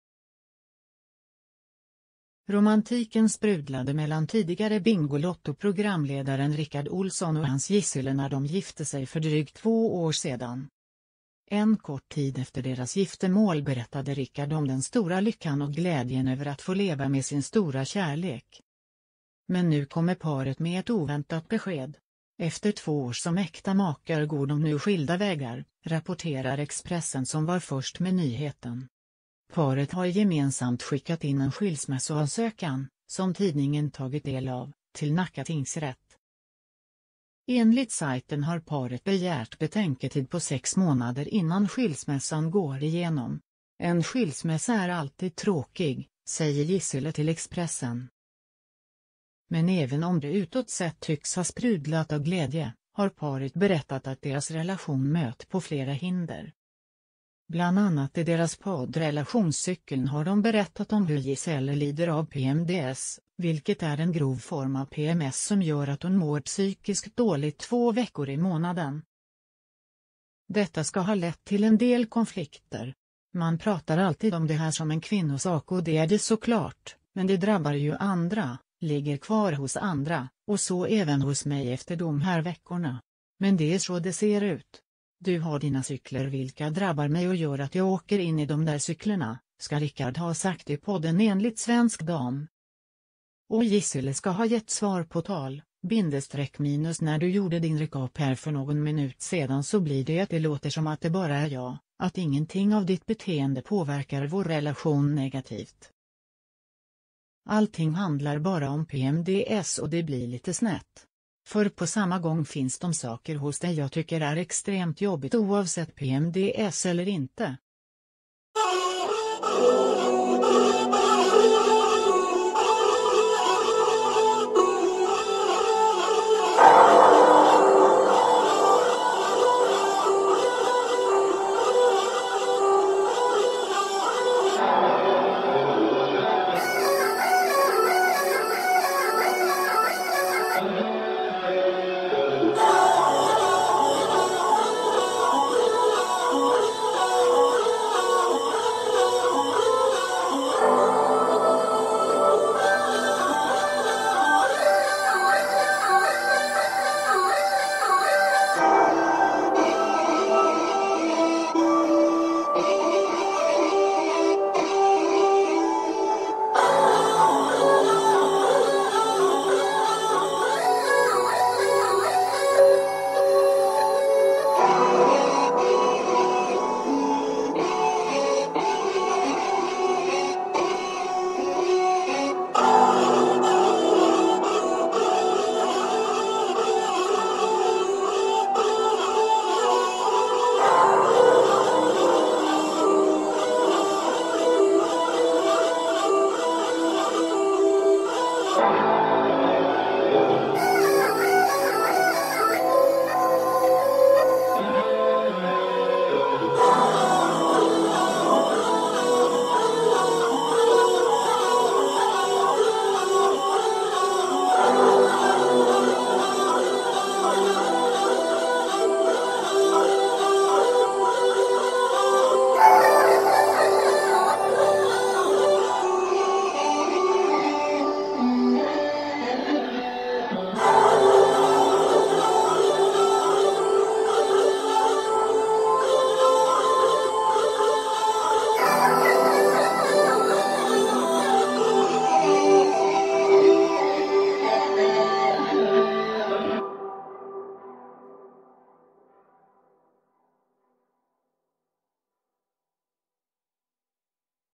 Rickard Olsson och hustrun Giséle skiljer sig – efter 2 år som gifta
Kärlekssagan är över.

För två år sedan sa folkkära programledaren Rickard Olsson ja till sin Giséle Essé.

Nu bekräftar paret beslutet.

– En skilsmässa är alltid tråkig, säger Giséle Essé Olsson till Expressen.